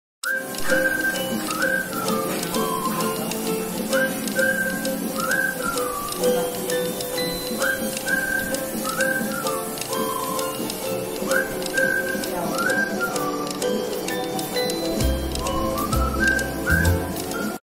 First ride on the Pram | Baby in Pram
Suruchi first time taking the ride of pram.